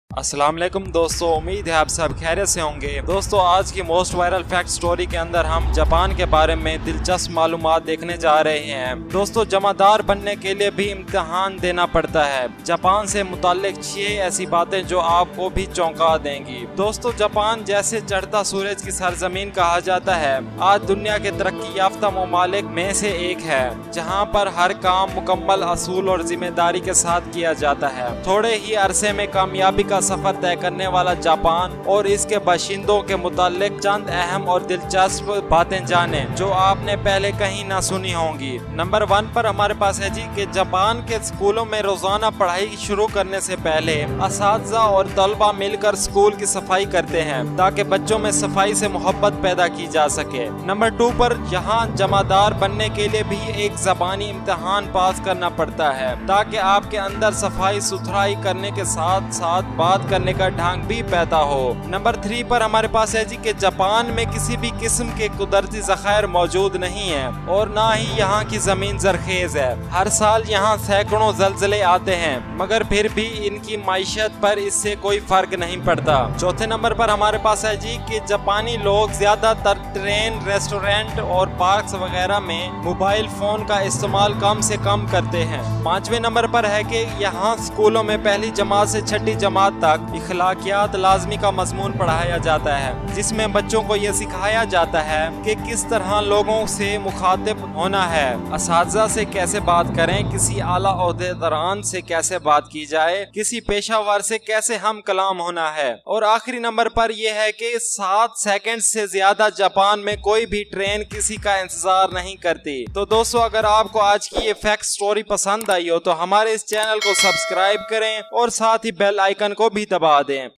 6 Things About the Japanese That Will Surprise You | جاپانیوں سےمتعلق6ایسی باتیں جوآپ کوچونکادیںگی
Friends Japan is called the "Land of the Rising Sun". Today it is one of the most developed countries in the world. Where everything is done with full principle and responsibility. Here are some important and interesting things about Japan and its people who have been on a successful journey in a short period of time that you may not have heard before.